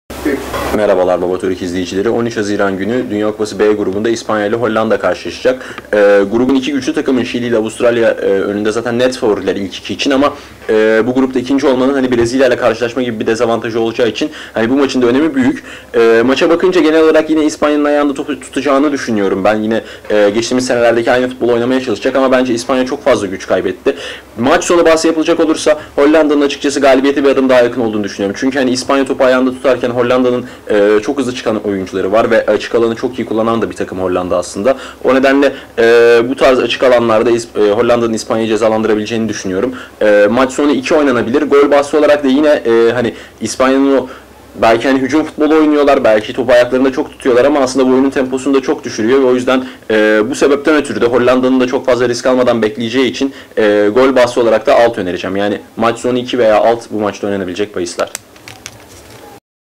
Babatorik İspanya Hollanda 13 Haziran 2014
Güney Dündar anlatiyor 13 Haziran 2014 İspanya Hollanda İspanya aynı tas aynı hamam